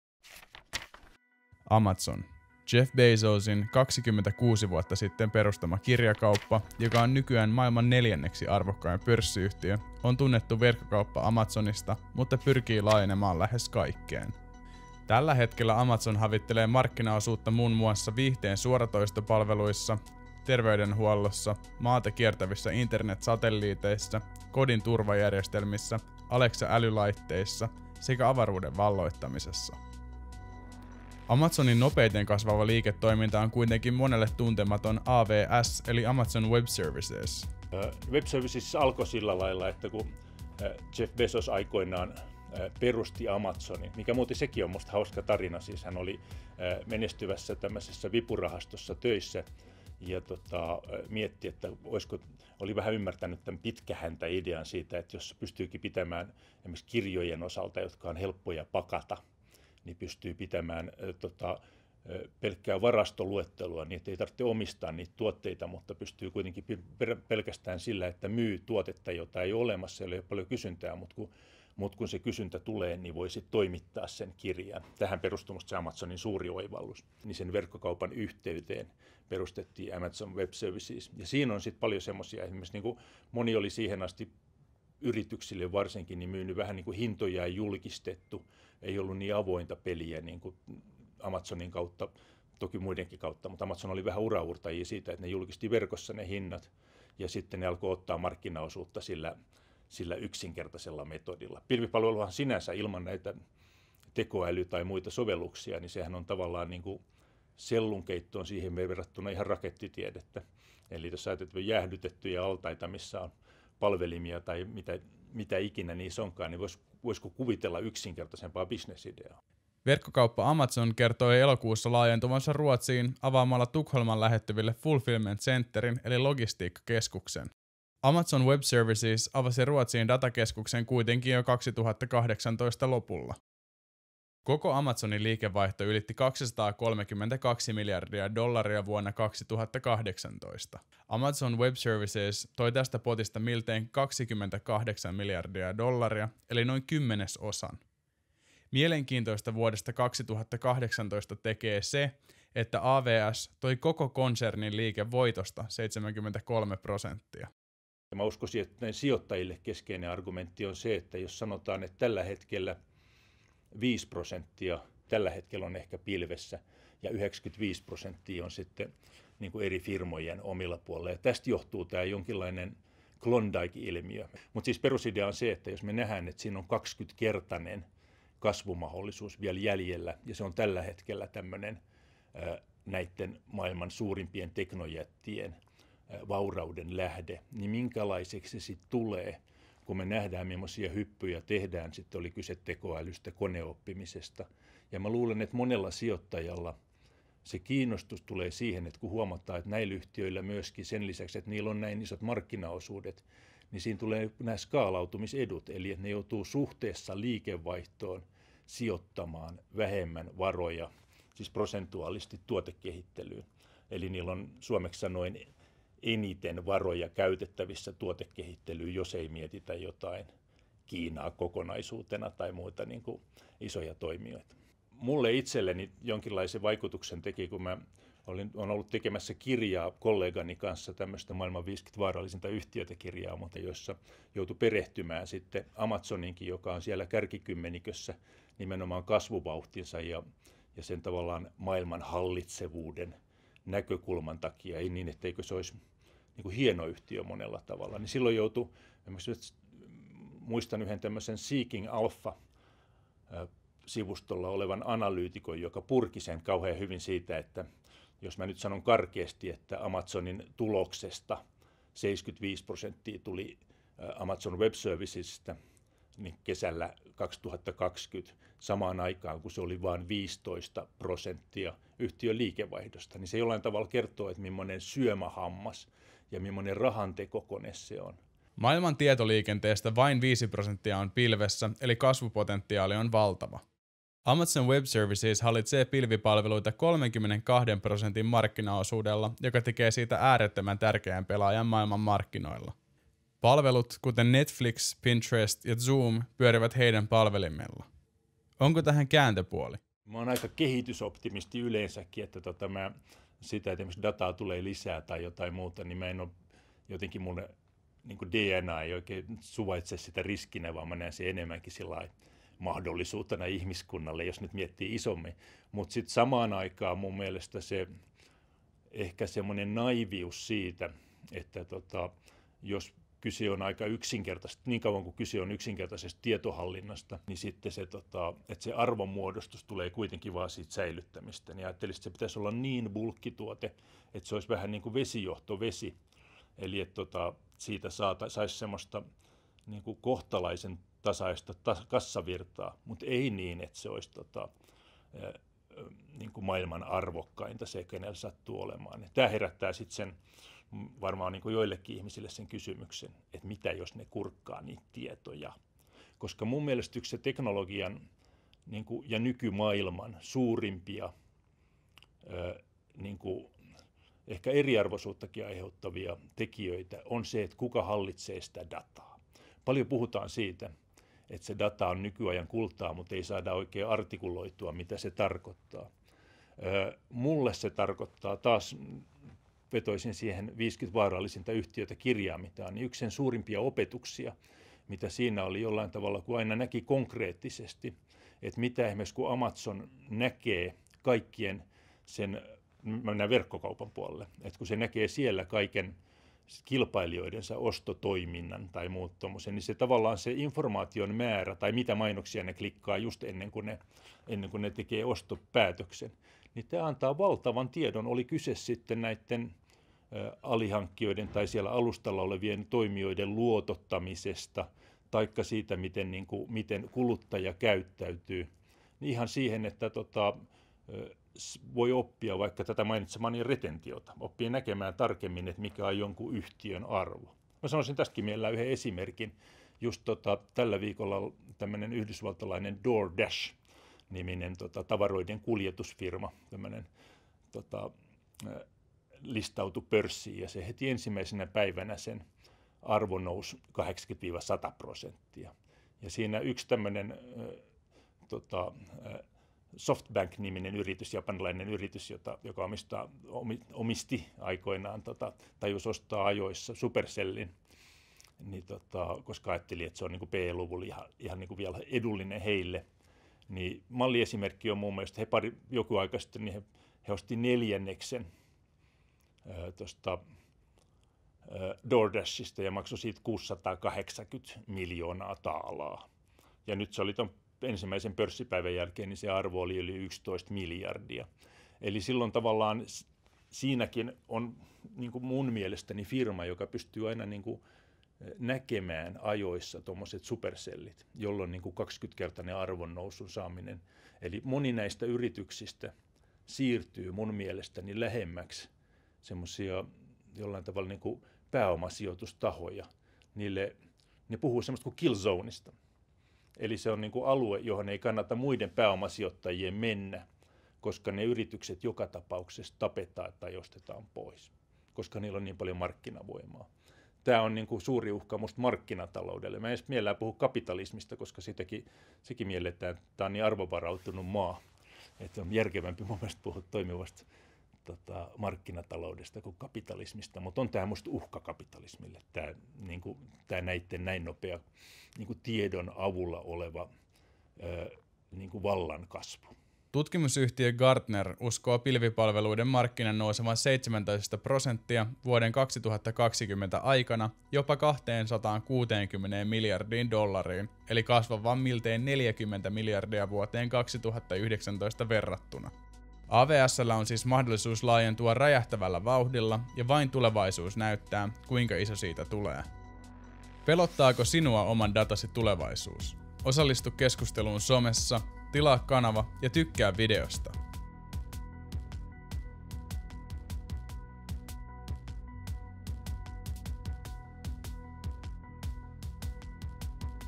Amazon on maailman vaarallisin yritys? – ja näin se kasvaa vielä vaarallisemmaksi
Amazon Web Services on maailman suurin pilvipalveluyritys, noin kaksi kertaa suurempi kuin toiseksi suurin kilpailijansa. Vaikka  AWS tuo Amazon konsernin liikevaihdosta alle viidenneksen, sen osuus konsernin liikevoitosta on ollut viime vuosina jopa yli 70 prosenttia. Amazon pystyy siis rahoittamaan verkkokauppansa ja muiden uusien liiketoimiensa kasvua Amazon Web Servicesin tuloksella.

Maailman 50 vaarallisinta yhtiötä-kirjan kirjoittanut Helsingin Sanomien taloustoimittaja Juha-Pekka Raeste kertoo videolla miten Amazon Web Services vaikuttaa Amazoniin ja miten teknologiayhtiöt voivat vaikuttaa maailmaan.